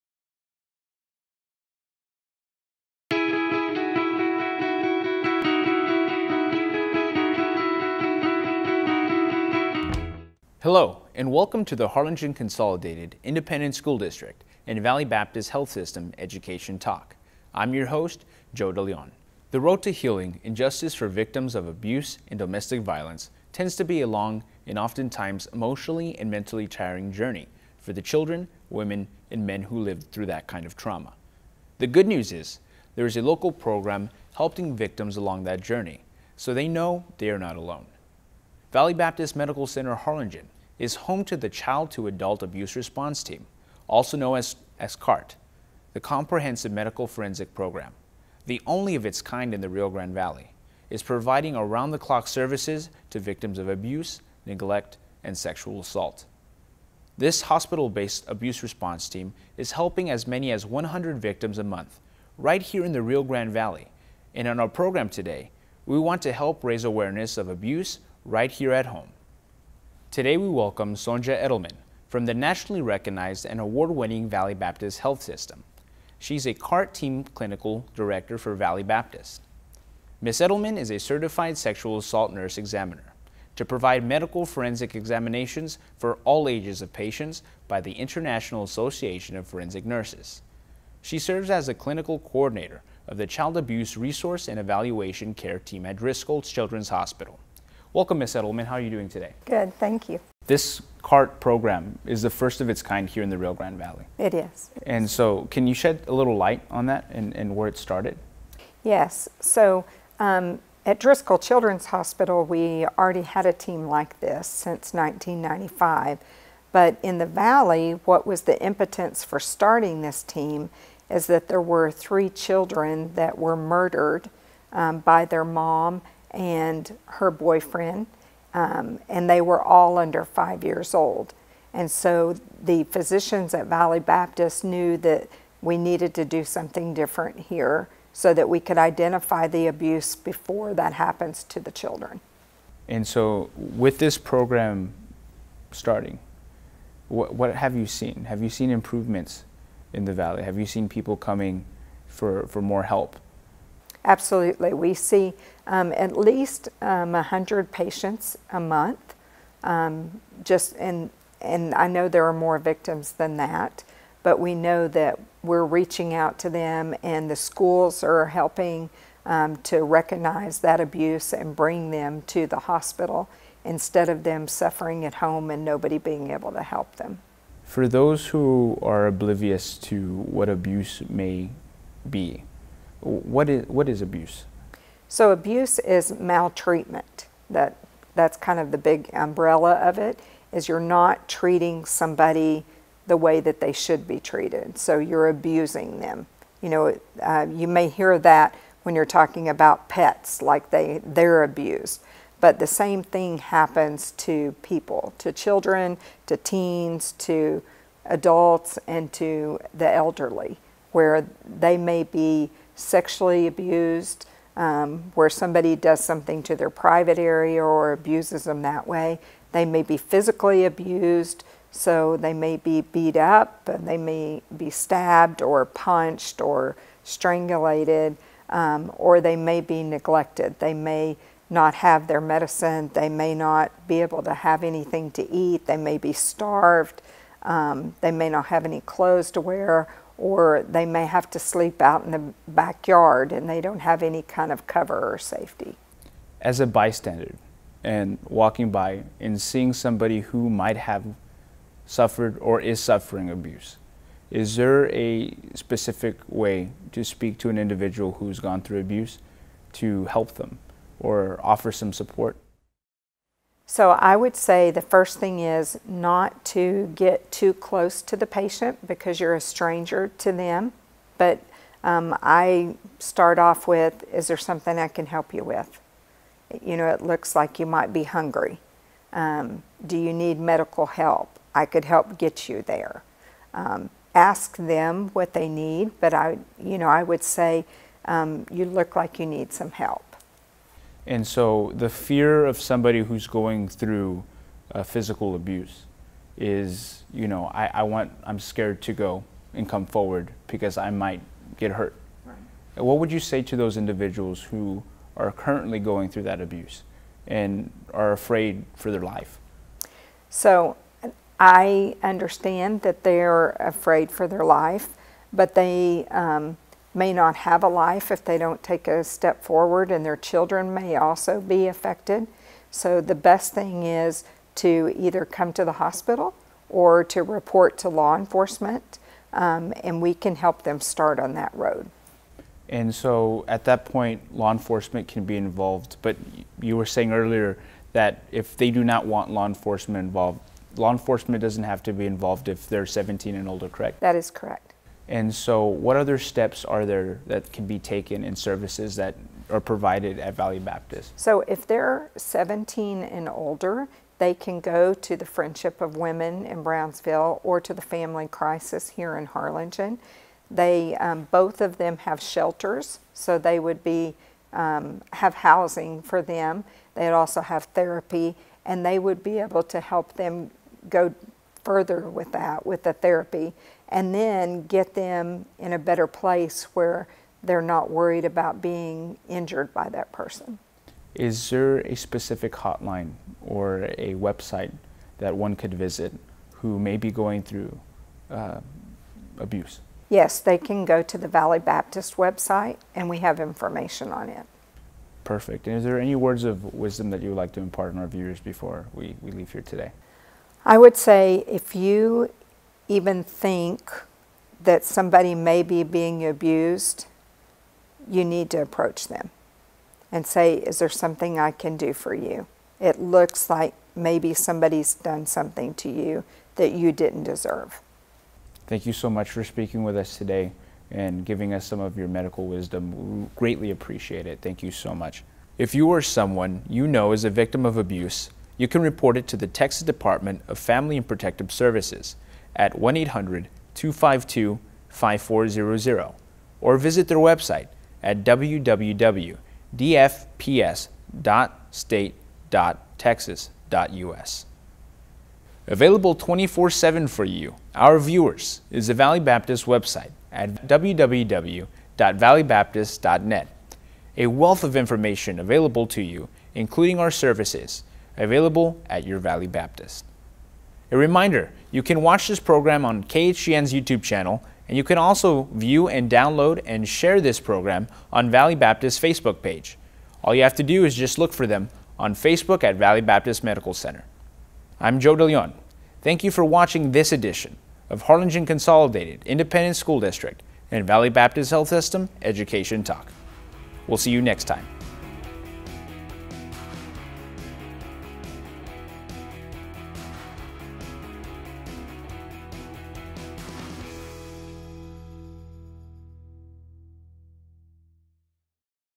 HCISD & VBMS Education Talk
On this edition of Education Talk we welcome Sonja Eddleman from Valley Baptist Medical Center in Harlingen. She discusses CAART: helping victims of abuse and domestic violence.